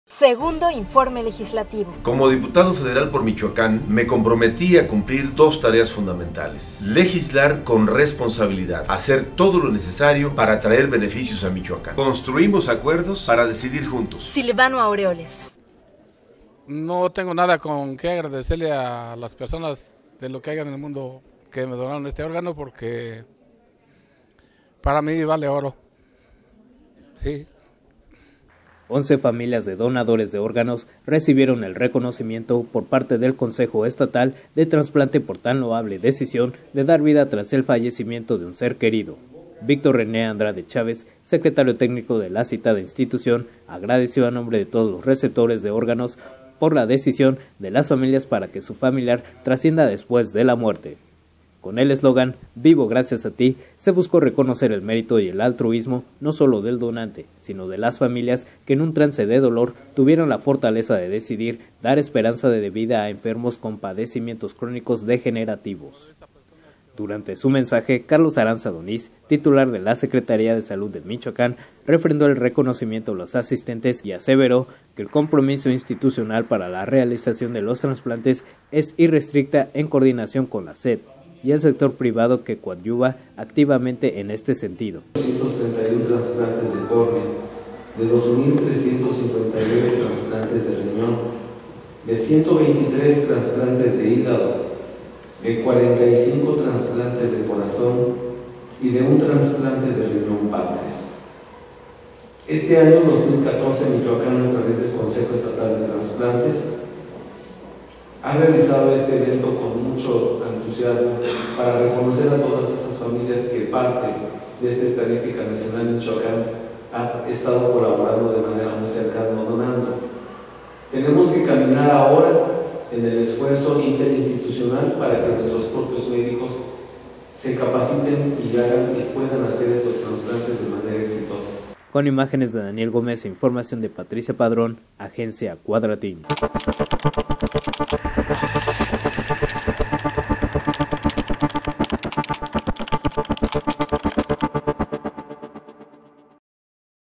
Reconocen a 11 familias de donadores de órganos
MORELIA, Mich., 4 de diciembre de 2014.-Once familias de donadores de órganos recibieron el reconocimiento por parte del Consejo Estatal de Trasplantes por tan loable decisión de dar vida tras el fallecimiento de un ser querido.